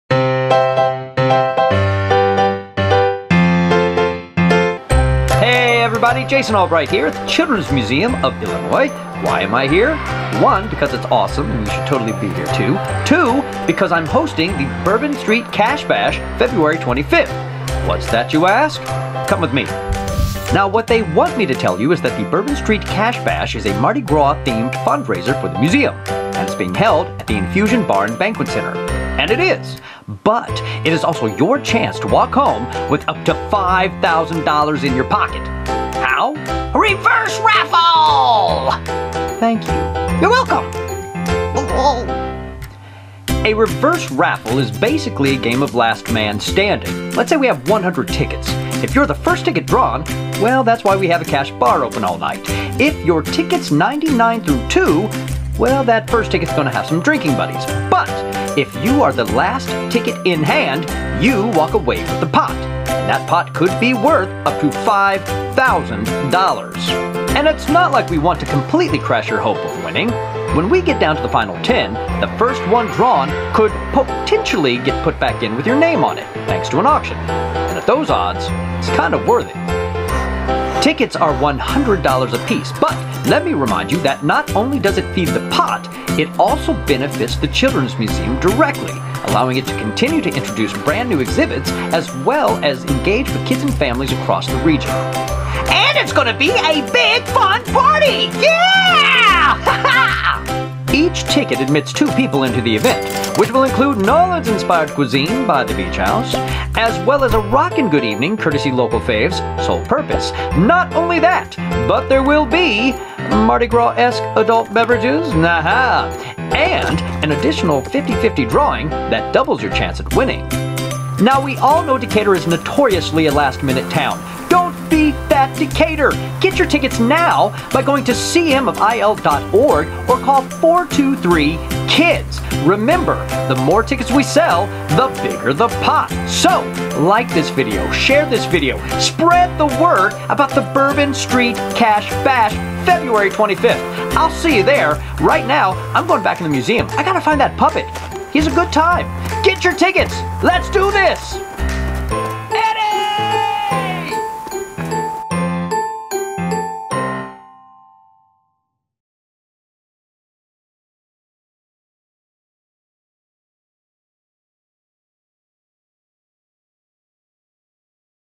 CMofIL Bourbon Street Cash Bash
Produced this vid for the Children's Museum of Illinois QUICK to help spread the word and sell tickets for their fundraising event, the Bourbon Street Cash Bash. And I mean quick-- we filmed the Monday before the Saturday event! Mission accomplished. Ticket sales indeed upticked, meaning an extremely well-attended event. Fun note: the museum's Executive Director, Amber Kaylor, was my camera operator for this-- did I mention it was a quick turnaround?!? She did a fantastic job, especially considering all the additional commotion going on at the museum that day!